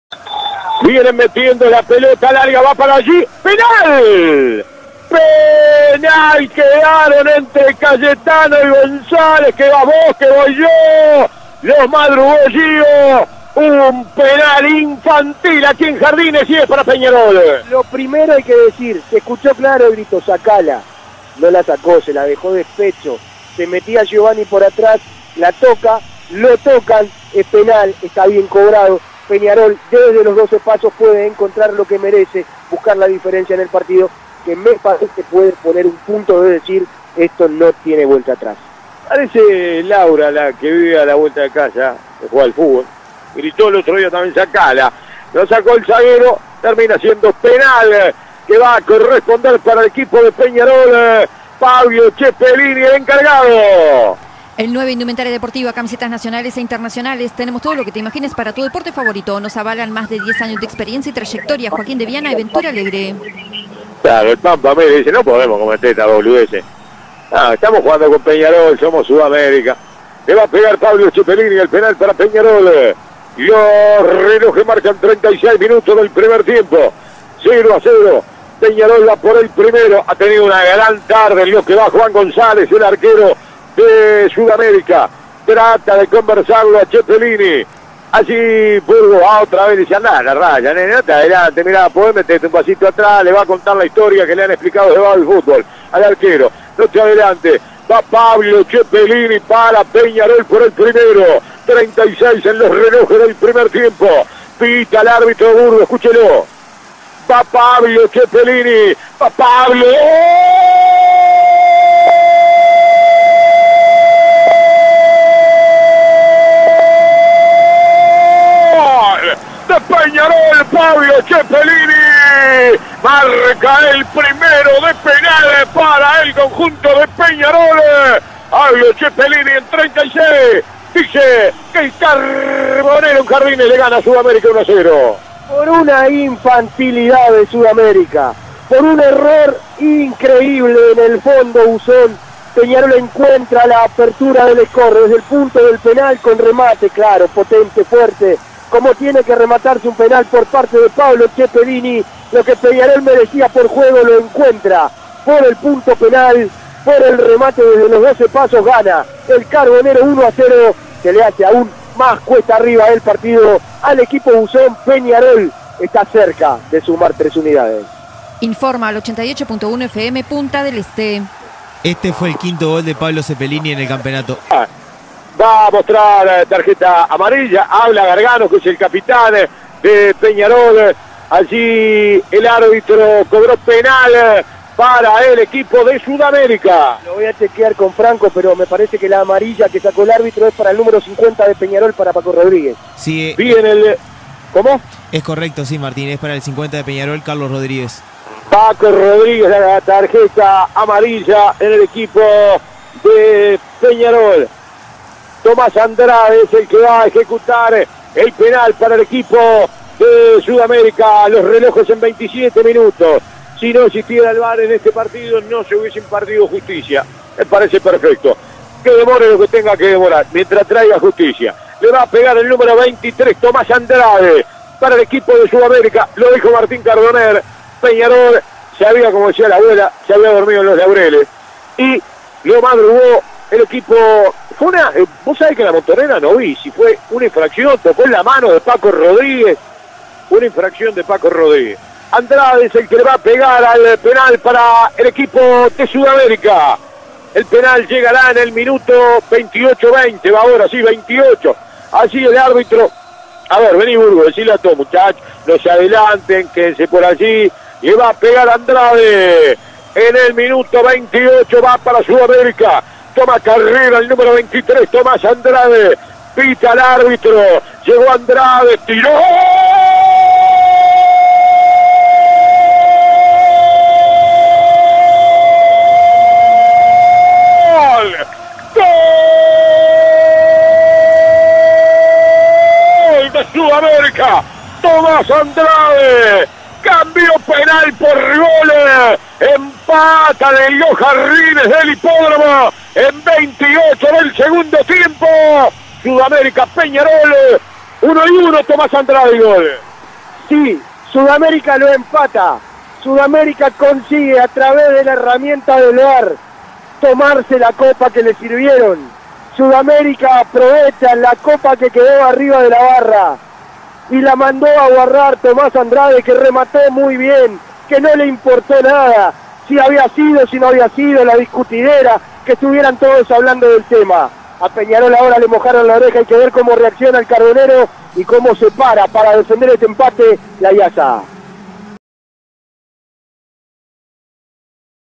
Sud América 1 Peñarol 1 - Apertura 2021 (por Luis Morales)
Los goles del partido, por Luis Morales y Martín Cardoner para 88.1 FM Punta del Este.